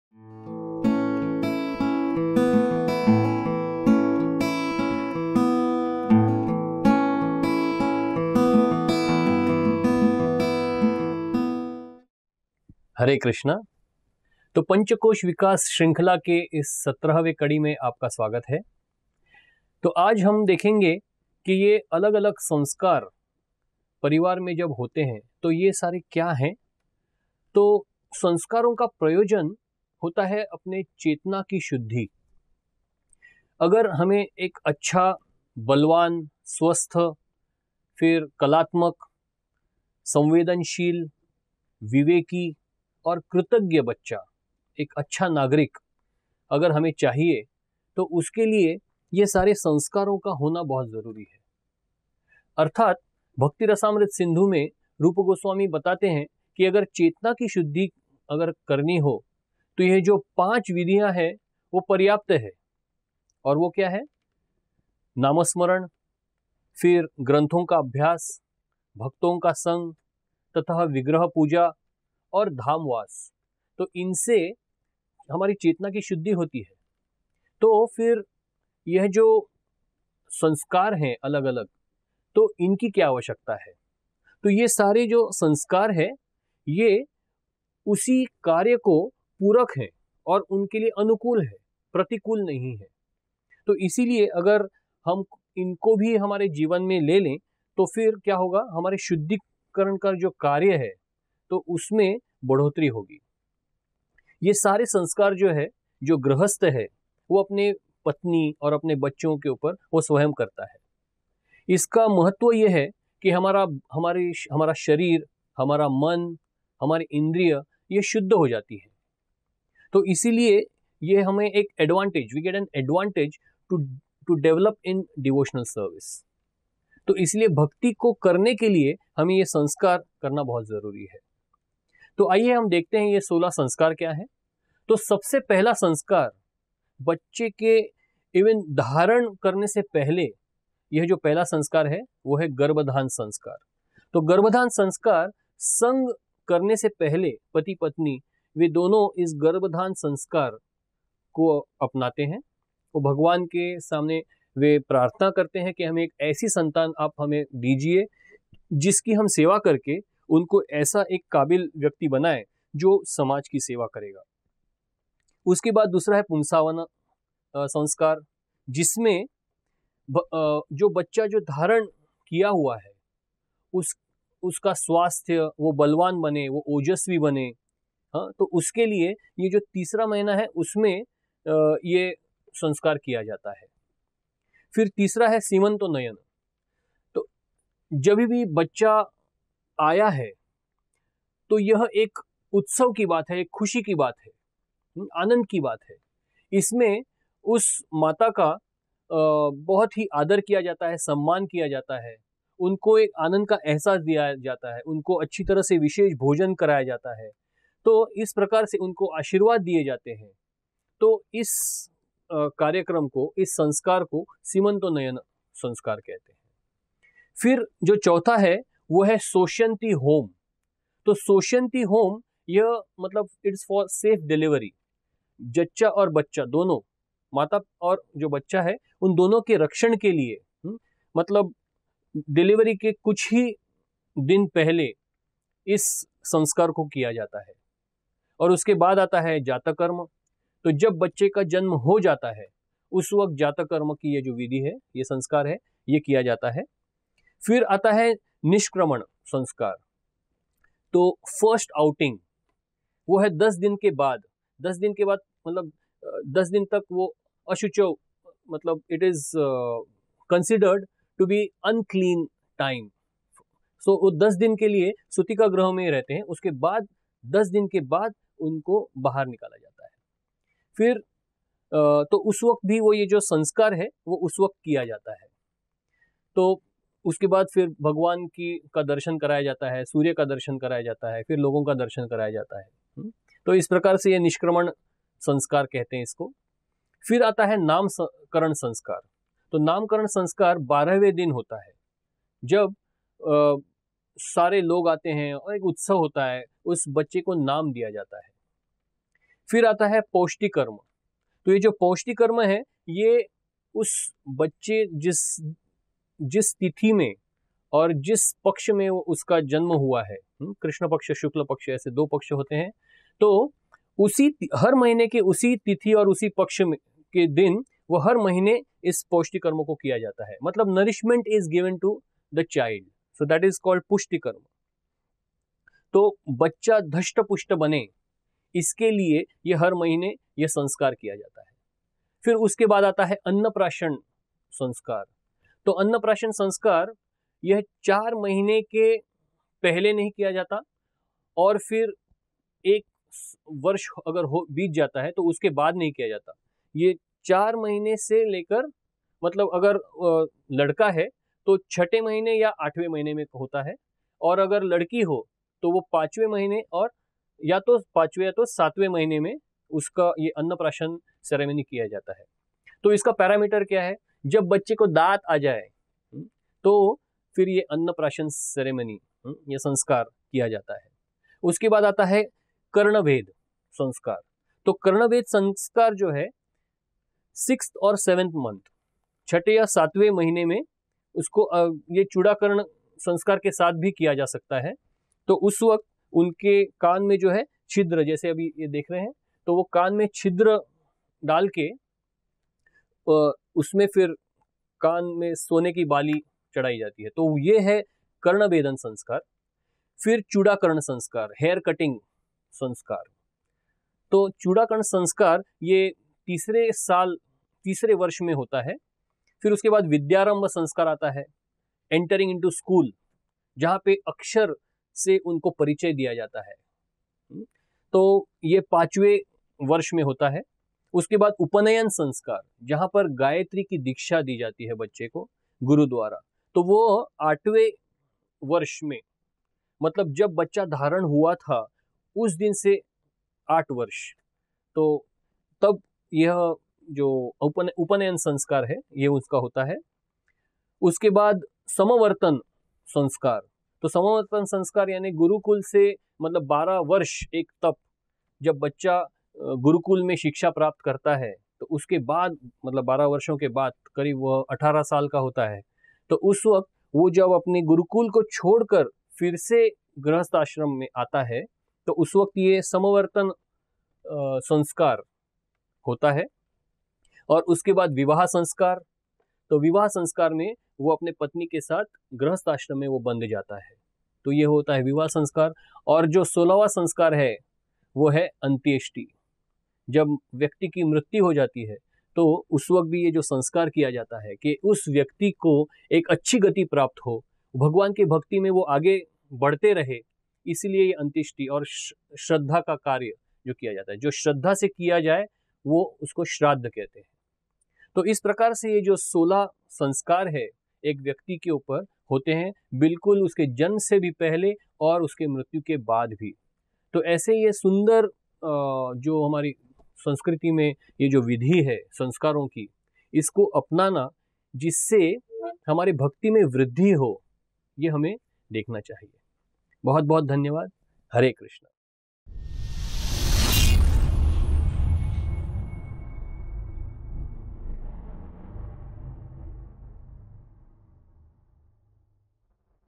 Episode 17 - सोलह संस्कार | Panchkosh vikas - श्रीमान रामशरण दास
योग की धारणा के अनुसार मानव का अस्तित्व पाँच भागों में बंटा है जिन्हें पंचकोश कहते हैं। ये कोश एक साथ विद्यमान ...